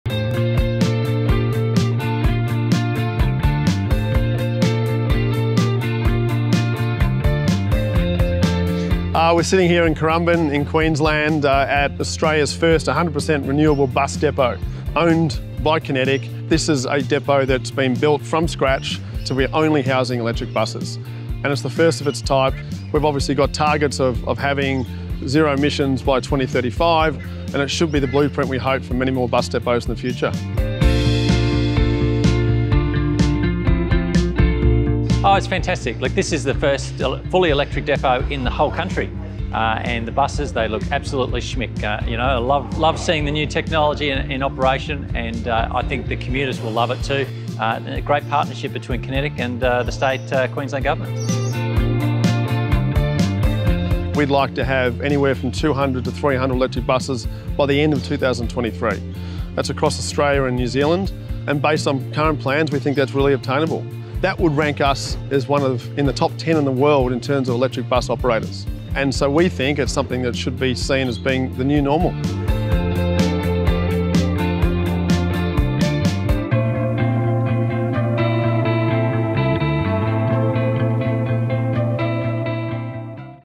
Kinetic powers Gold Coast electric bus evolution
Ten new battery electric buses (BEB) have been unveiled on the Gold Coast as Kinetic supports the Queensland Government in electrifying the state’s public transport network.

Take a look behind the scenes of Kinetic’s depot in Currumbin, which is Australia’s first 100 per cent electric bus depot.

The zero emission vehicles operate the 777 route connecting the Gold Coast Airport to the heart of the city and are among 20 fully electric vehicles Kinetic is rolling out across Queensland in 2022.

Kinetic is Australasia’s largest owner and operator of battery electric buses (BEB) with its current fleet expected to grow to 300 by the end of 2023. Kinetic’s ambition is to reach 100 per cent net zero emissions by 2035.